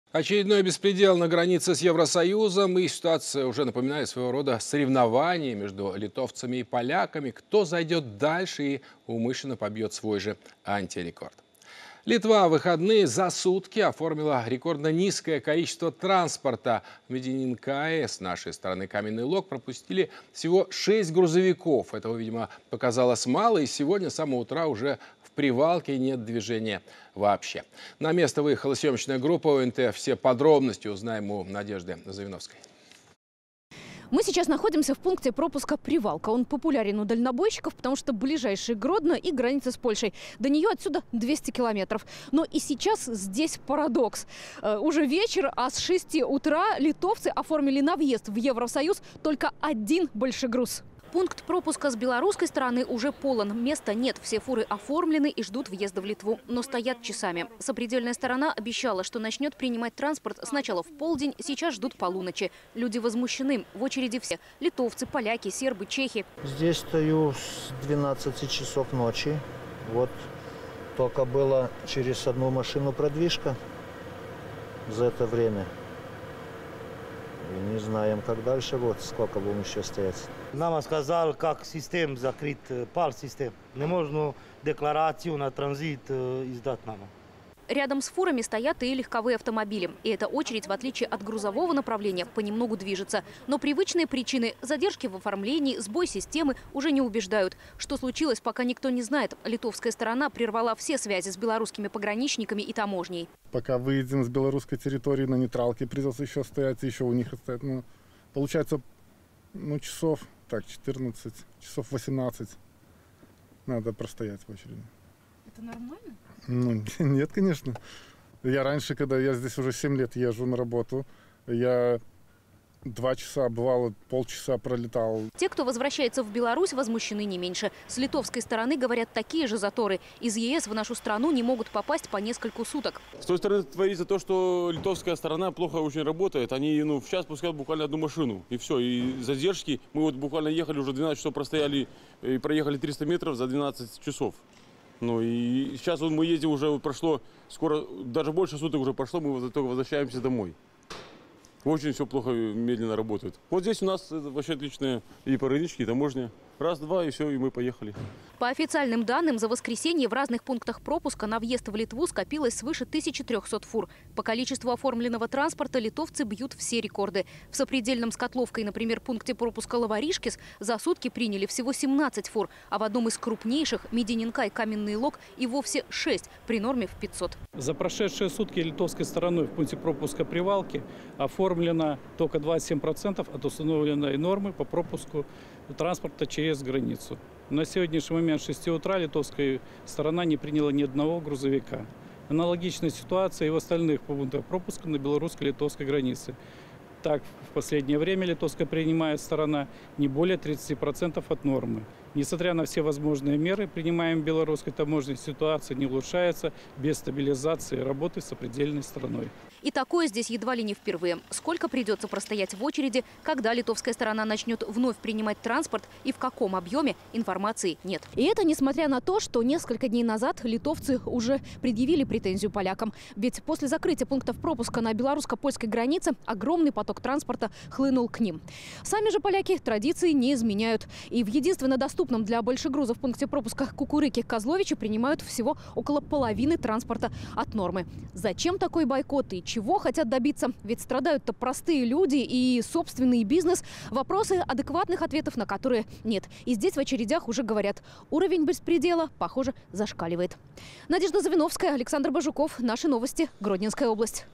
Беспредел на границе с ЕС: более 1,3 тыс. грузовиков застряло на въезде в Литву
Очередной беспредел на границе с Евросоюзом. И ситуация уже напоминает "соревнование" между литовцами и поляками, кто зайдет дальше и умышленно побьет свой же антирекорд.